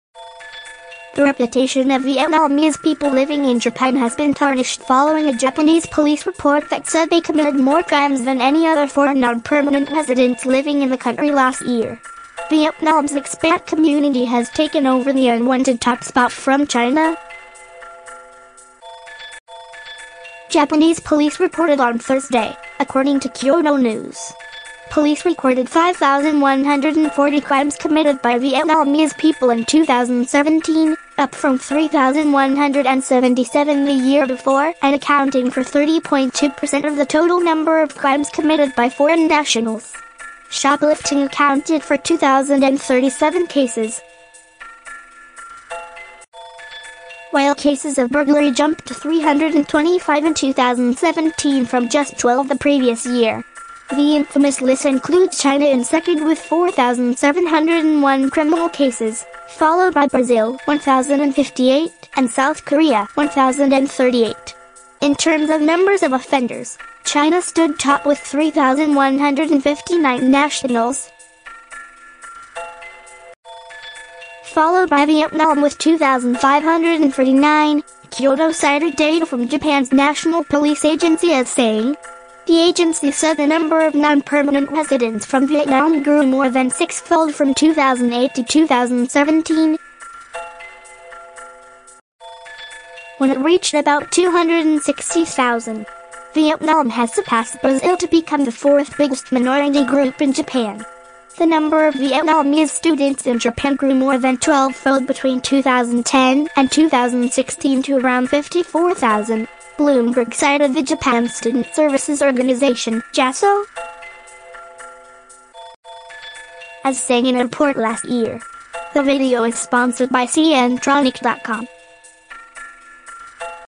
Vietnamese crime rate in Japan stands at unwanted all-time high: report
The reputation of Vietnamese people living in Japan has been tarnished following a Japanese police report that said they committed more crimes than any other foreign non-permanent residents living in the country last year.
Vietnam's expat community has taken over the unwanted top spot from China, Japanese police reported on Thursday, according to Kyodo News.
Police recorded 5,140 crimes committed by Vietnamese people in 2017, up from 3,177 the year before and accounting for 30.2 percent of the total number of crimes committed by foreign nationals.
Shoplifting accounted for 2,037 cases, while cases of burglary jumped to 325 in 2017 from just 12 the previous year.
The infamous list includes China in second with 4,701 criminal cases, followed by Brazil (1,058) and South Korea (1,038).
In terms of numbers of offenders, China stood top with 3,159 nationals, followed by Vietnam with 2,549, Kyodo cited data from Japan’s National Police Agency as saying.
The agency said the number of non-permanent residents from Vietnam grew more than six-fold from 2008 to 2017, when it reached about 260,000.
Vietnam has surpassed Brazil to become the fourth biggest minority group in Japan.
The number of Vietnamese students in Japan grew more than 12-fold between 2010 and 2016 to around 54,000, Bloomberg cited the Japan Student Services Organization (JASSO) as saying in a report last year.

👉 Construction execs get up to 2 years in jail over ruptures to notorious Hanoi water pipeline
👉 Vietnamese woman